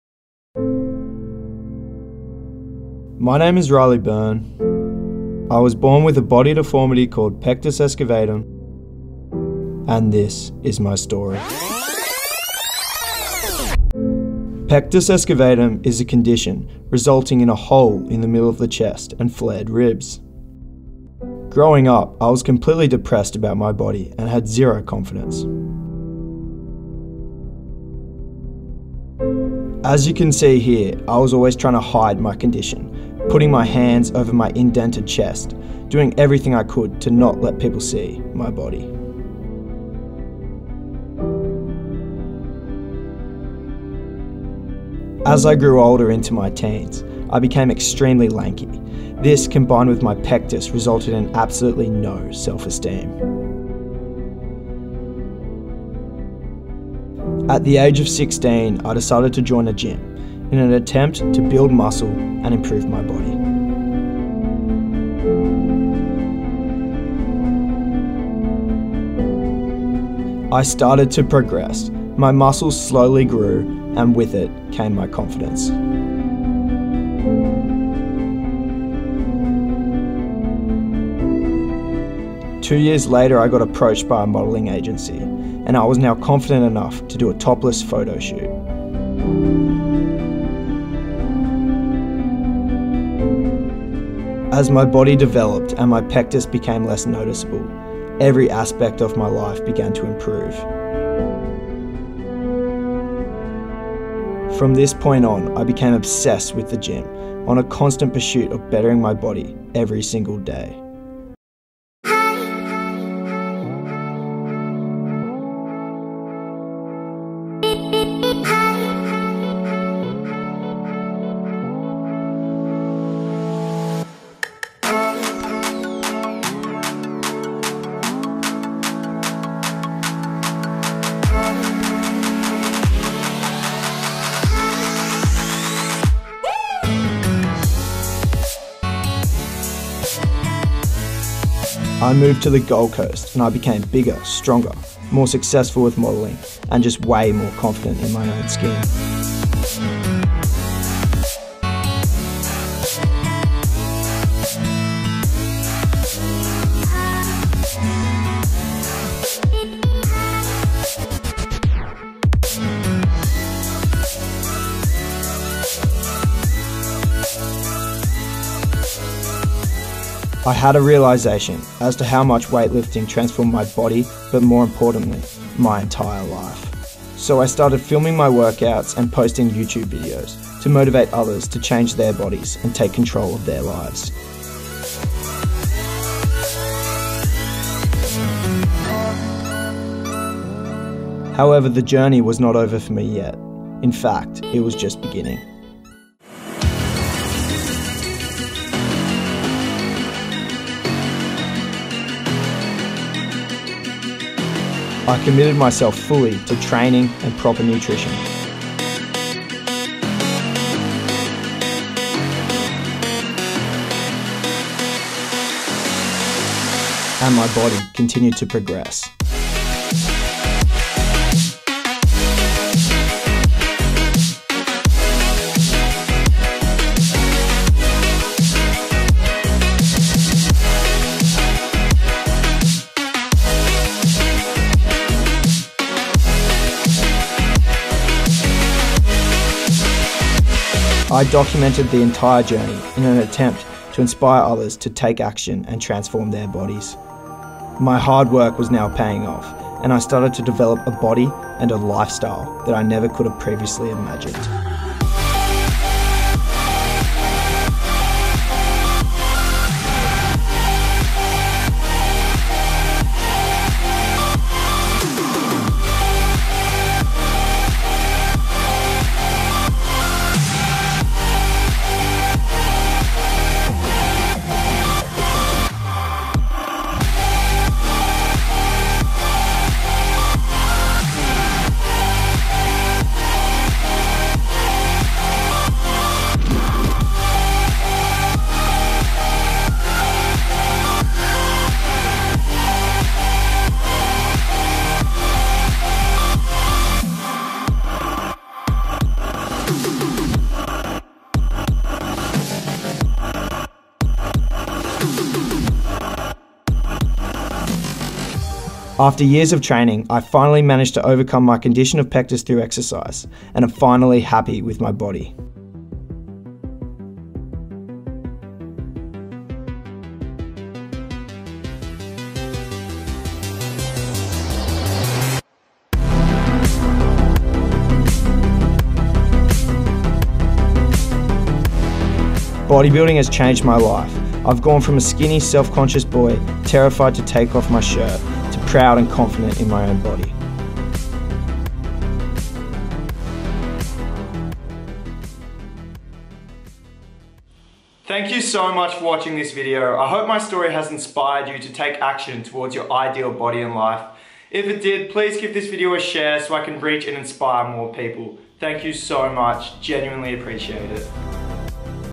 Pectus Excavatum (Sunken Chest) Transformation - Riley Byrne
I ( Riley Byrne )was born with Pectus Excavatum and managed to fix my condition through exercise & bodybuilding without surgery and I want to help others do the same.

A  typical person with Pectus Excavatum has tight chest muscles, poor upper body strength and bad posture. Exercise can correct all of these problems, thus greatly improving the appearance of your body.